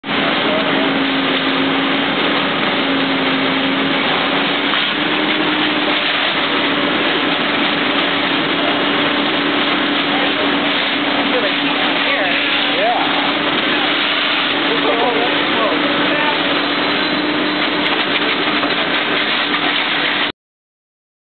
MVFD Exercise Fully Engulfed
A  MVFD training exercise. Firefighters from other cities are attending to observe the development of a flashover and its subsequent dousing (that's why so many are standing around taking pics and video like me instead of putting it out).